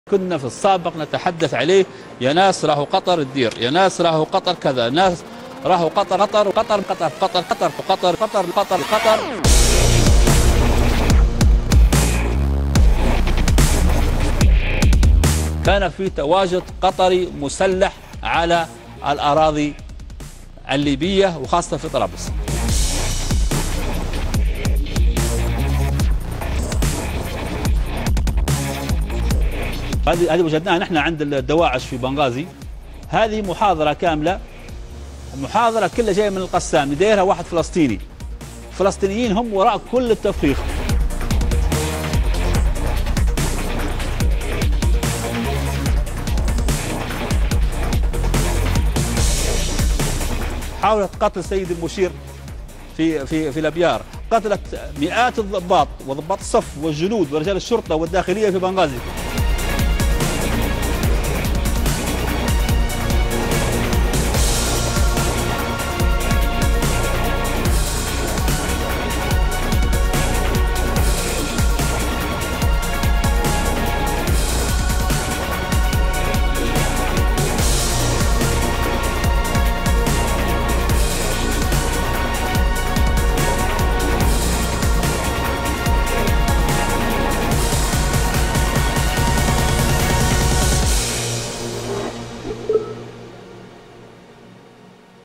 المتحدث باسم حفتر يتهم قطر وحماس بممارسات "إرهابية" في ليبيا
اتهم الناطق الرسمي باسم قوات خليفة حفتر كلاً من قطر وحماس بالتورط في عمليات إرهابية داخل ليبيا خلال مؤتمر صحفي عرض فيه وثائق وتسجيلات قال إنها دلائل على صحة ادعاءاته. وتبين لاحقاً أن التسجيلات تعود لتدريبات كتائب القسام في غزة ومتوفرة على الإنترنت.